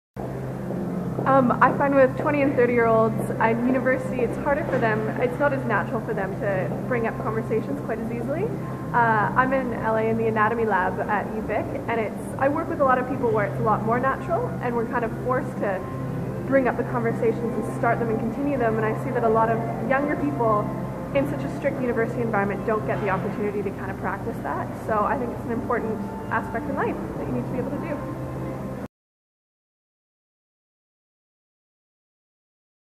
campus conversation
An Lab Assistant at UVic sees first hand the challenges university students face in  unstructured situations in making conversation.